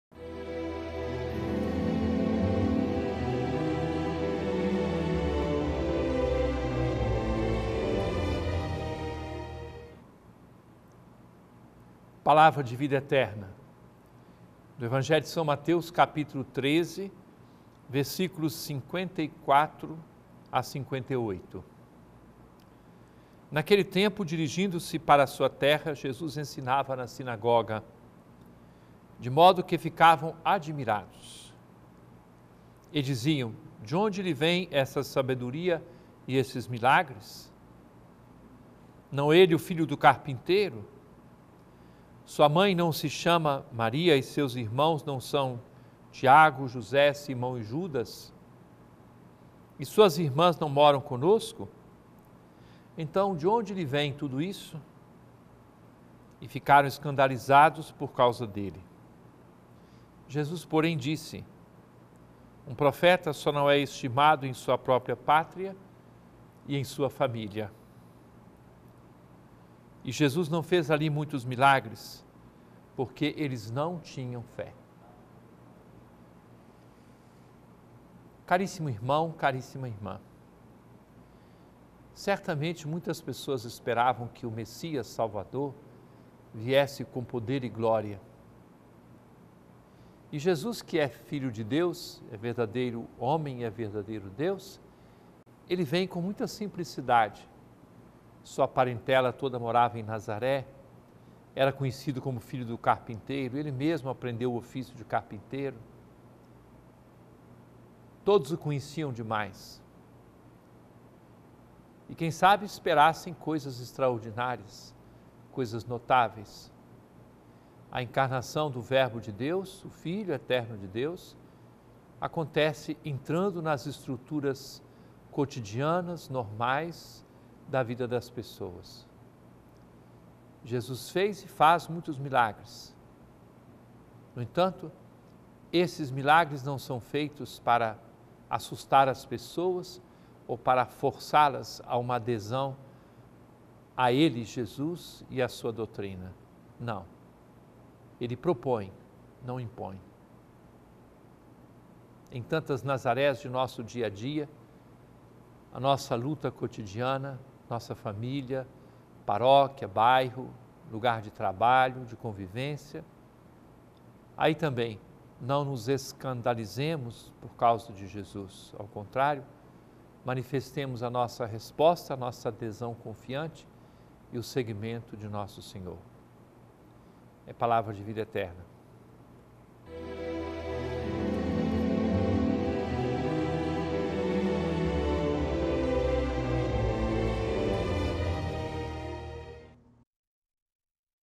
Palavra de Vida Eterna | São Mateus 13,54-58 - 04/08/2017
Naquele tempo, 54dirigindo-se para a sua terra, Jesus ensinava na sinagoga, de modo que ficavam admirados. E diziam: “De onde lhe vem essa sabedoria e esses milagres? 55Não é ele o filho do carpinteiro? Sua mãe não se chama Maria, e seus irmãos não são Tiago, José, Simão e Judas? 56E suas irmãs não moram conosco? Então de onde lhe vem tudo isso?” 57E ficaram escandalizados por causa dele. Jesus, porém, disse: “Um profeta só não é estimado em sua própria pátria e em sua família!” 58E Jesus não fez ali muitos milagres, porque eles não tinham fé.